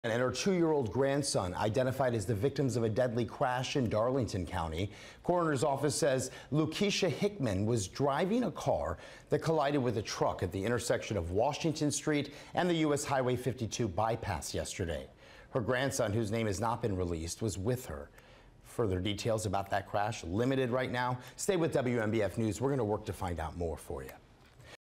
Coroner: Woman, 2-year-old grandson killed in Darlington County crash
A woman and her grandson were both killed after a crash in the Pee Dee on Thursday, according to an official.